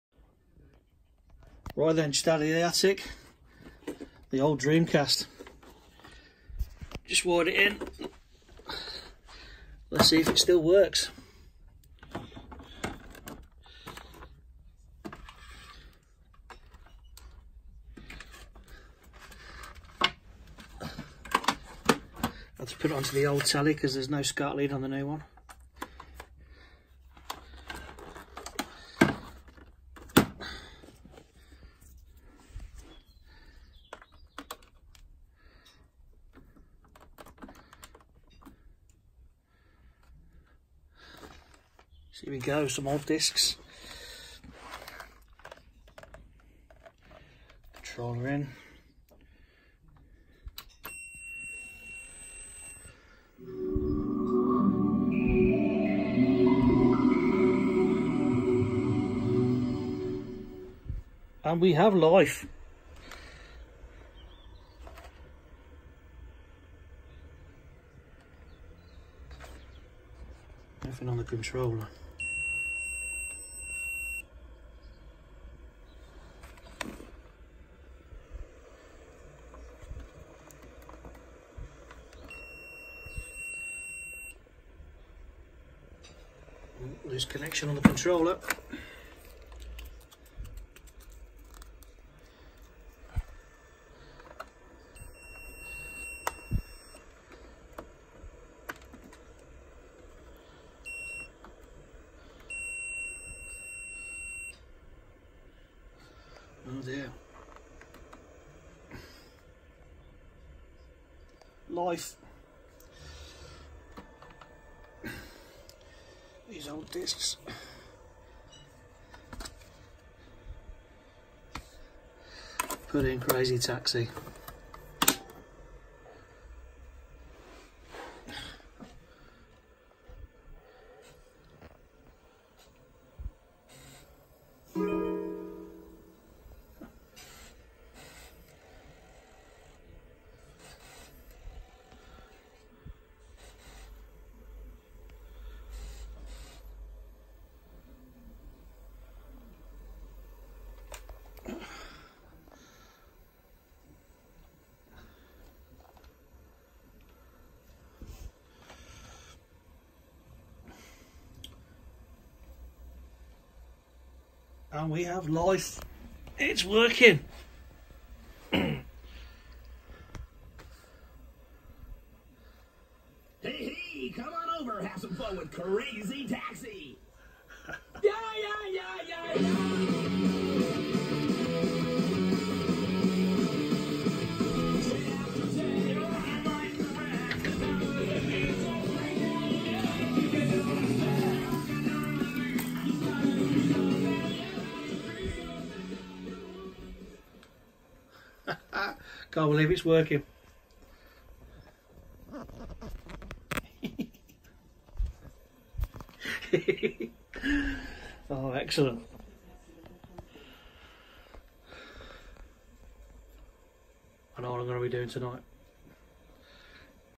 Will the Sega Dreamcast fire up.
Sega Dreamcast has been in the attic over 20 years and it still works.